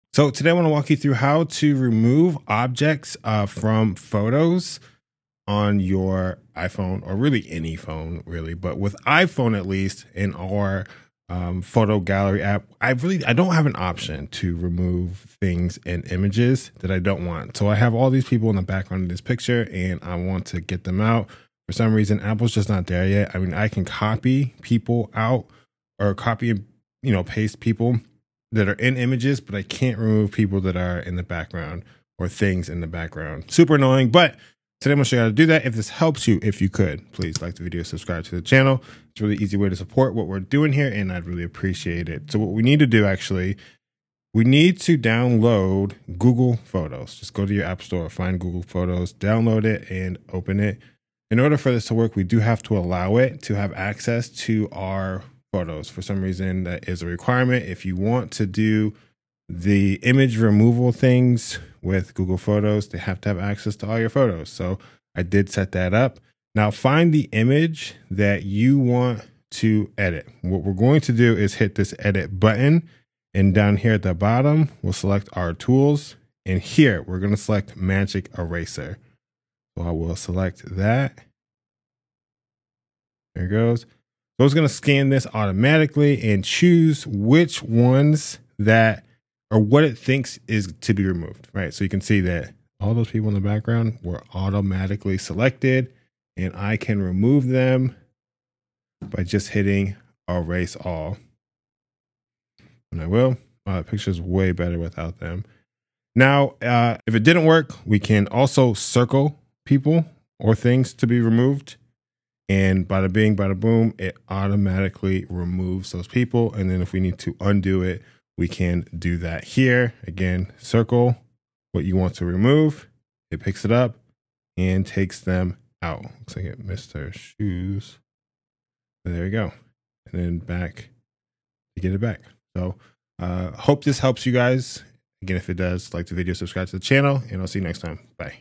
Remove Unwanted Objects from iPhone Photos Using Magic Eraser
Want to remove unwanted people or objects from your photos on an iPhone? While Apple doesn’t have a built-in tool yet, you can use Google Photos’ Magic Eraser feature to clean up your pictures effortlessly.

Steps to Remove Objects from Photos:

Download Google Photos from the App Store.
Allow access to your photo library.
Open the photo you want to edit and tap Edit.
Select Tools / Magic Eraser.
Google Photos will automatically detect objects to remove. Tap Erase All to clean up.
If automatic selection doesn’t work, simply circle the object you want to remove.
Save your edited photo.